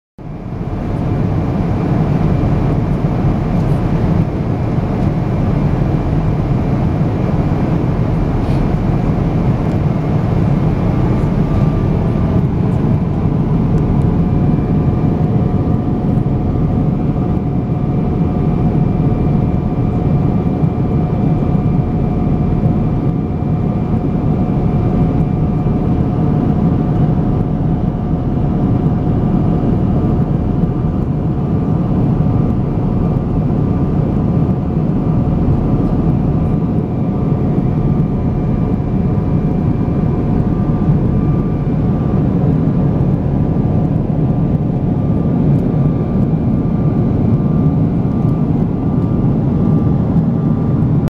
Flight Navigator (UZONE F5 Deluxe + GPS Essentials)
B787で測位に失敗したのでB777で再チャレンジしてみました。